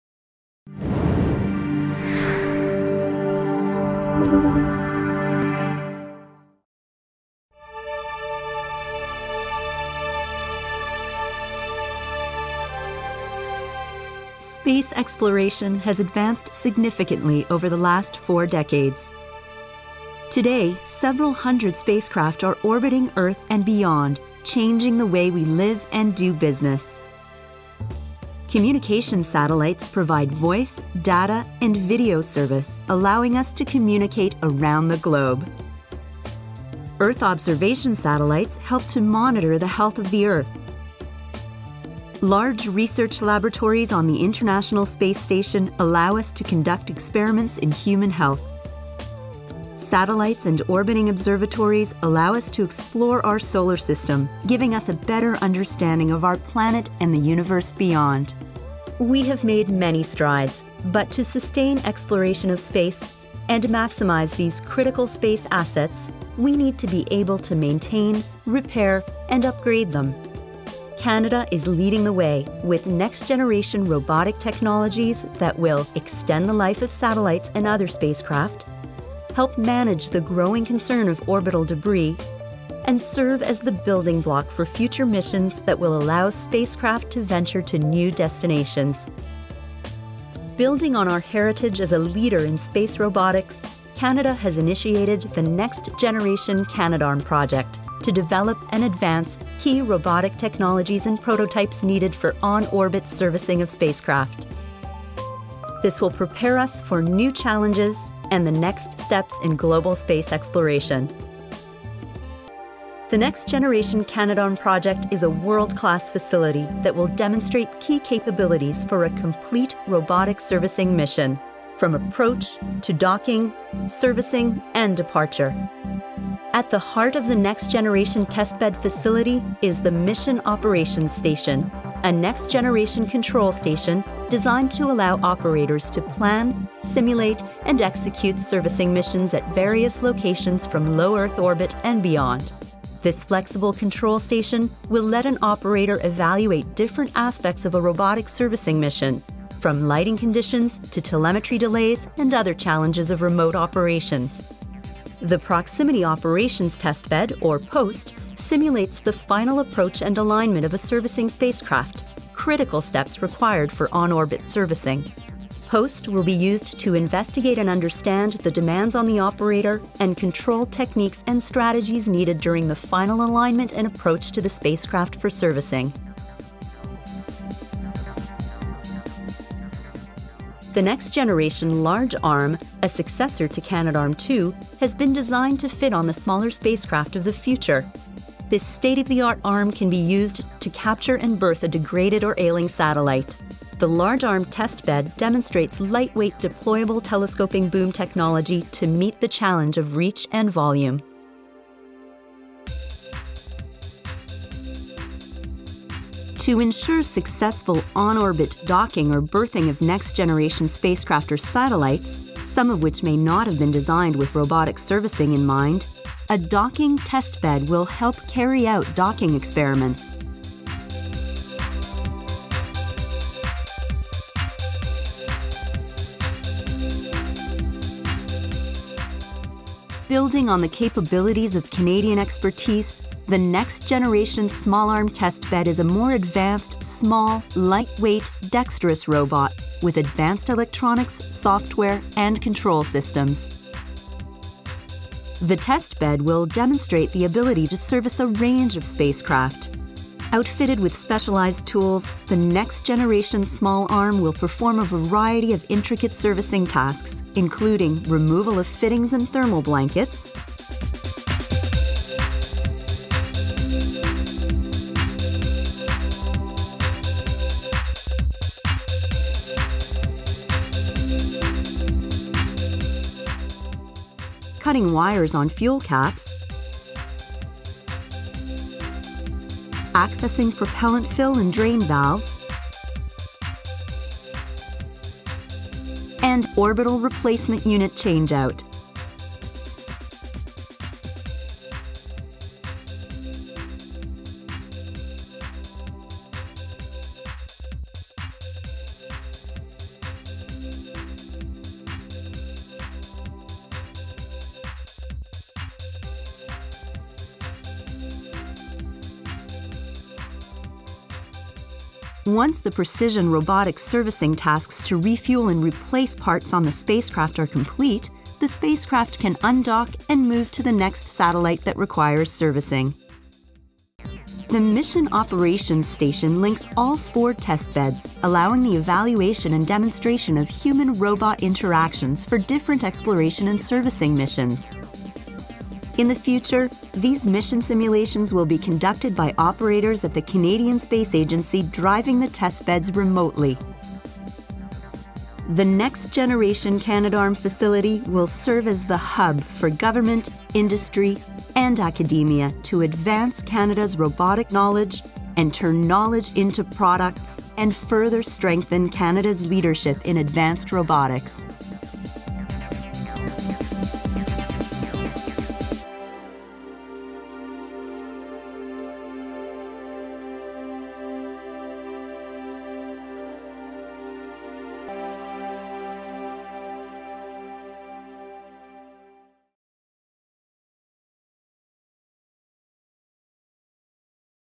A New Generation of Spacecraft Robot Arms is Coming | CSA Space Science HD
- new Canadian robotic technology is being designed to support future space missions, as well as servicing existing satellites. Please rate and comment, thanks!